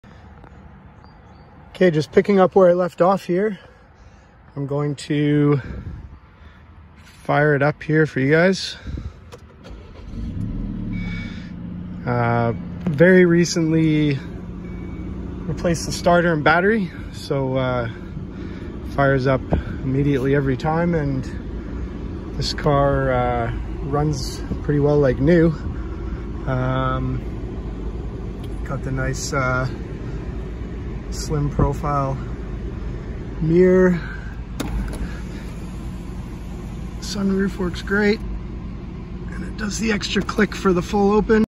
2017 Volvo V60 Polestar - walk around and drive (part 2)
Created for upcoming sale on  @Cars & Bids